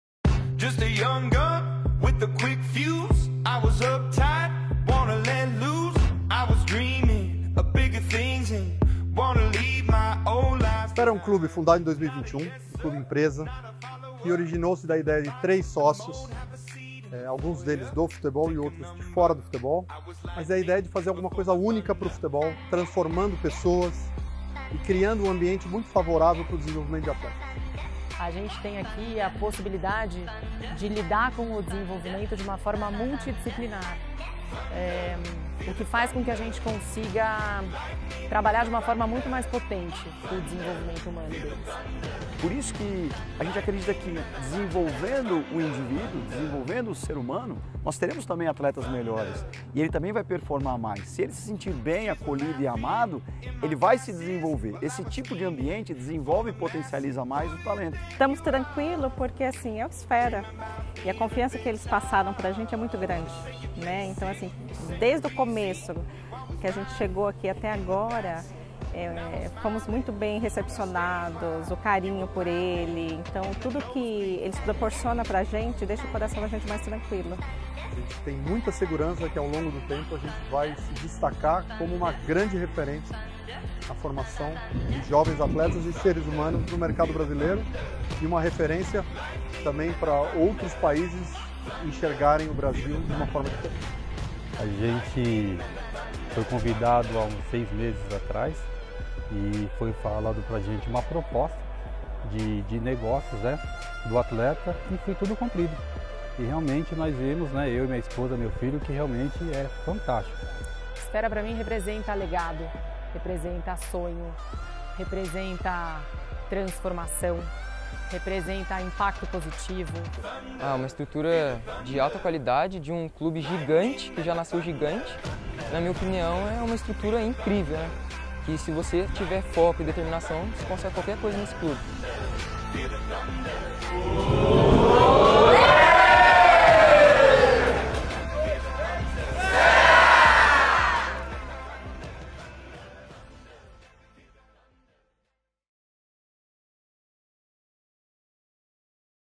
Sfera Futebol Clube
Vídeo realizado no Centro de Treinamento durante o acolhimento dos primeiros atletas do Sfera FC.